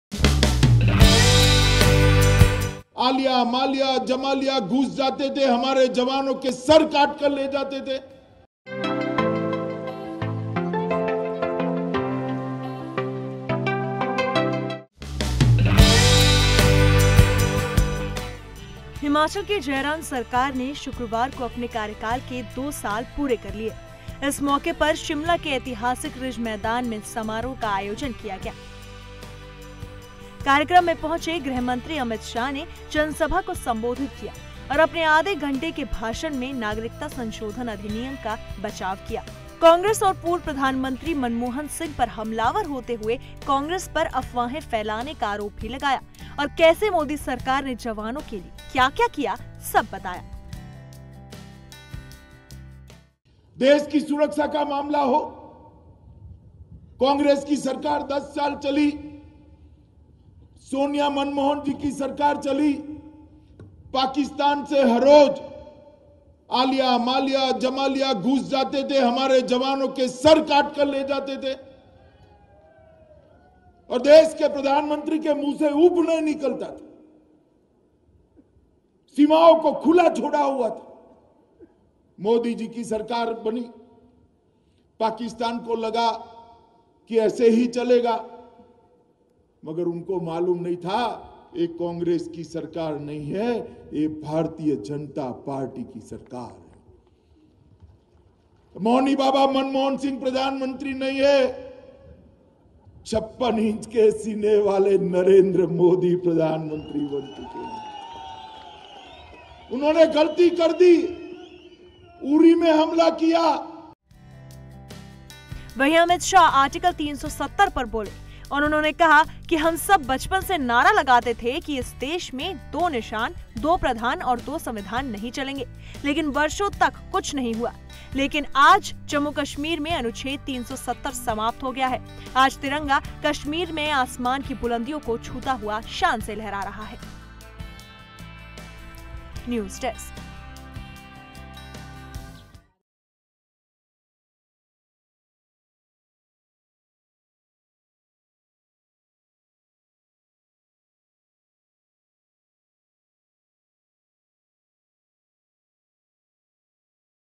Shimla: Congress पर गरजे Amit Shah, कहा- ये मौनी बाबा की नहीं, 56 इंच वाले Modi की सरकार है
हिमाचल की जयराम सरकार ने शुक्रवार को अपने कार्यकाल के दो साल पूरे कर लिए। इस मौके पर शिमला के ऐतिहासिक रिज मैदान मेंसमारोह का आयोजन किया गया। कार्यक्रम में पहुंचे गृहमंत्री अमित शाह ने जनसभा को संबोधित किया और  अपने आधे घंटे के भाषण में नागरिकता संशोधन अधिनियम (सीएए) का बचाव किया. कांग्रेस और पूर्व प्रधानमंत्री मनमोहन सिंह पर हमलावर होते हुए कांग्रेस पर अफवाहें फैलाने का आरोप भी लगाया..